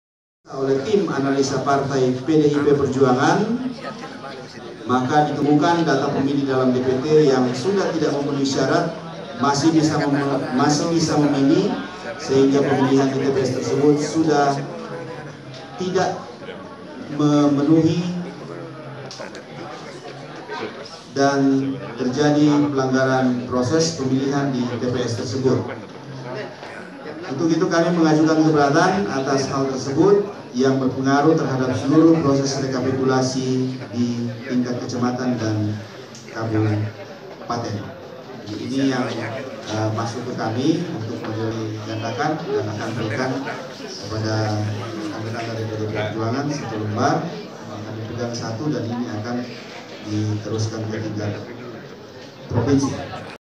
Video ; KPU Toraja Utara Membacakan Nota Protes PDIP Untuk Kecamatan Tondon
KPU Toraja Utara